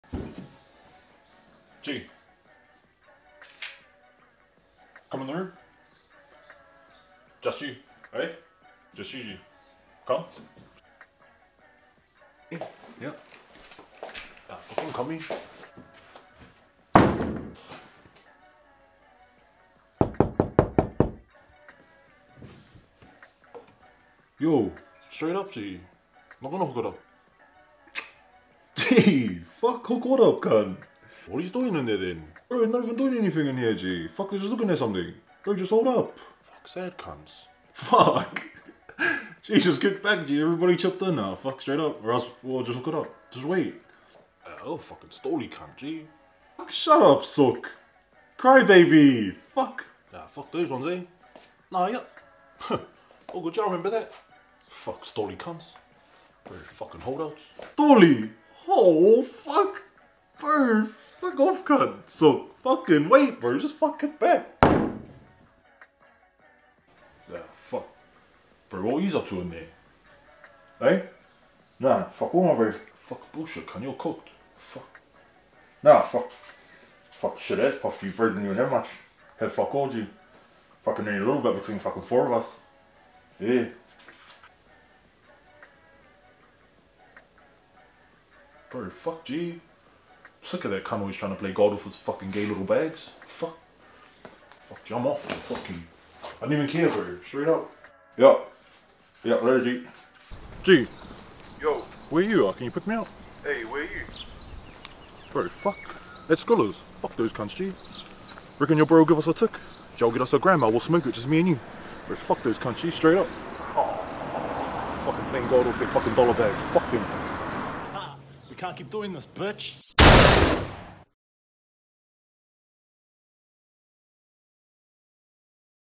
WHEN YOU DONT GET THE INVITE
The bro has a cry cause he doesn’t get the invite to go in the room 😂